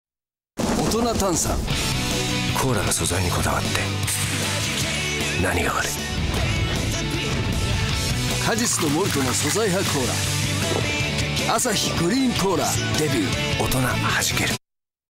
氷室京介 - アサヒ グリーンコーラ ＴＶＣＭ 「大人炭酸第一弾」篇
氷室京介出演! アサヒ飲料 グリーンコーラ　TV CM。2010年5月25日より放映開始。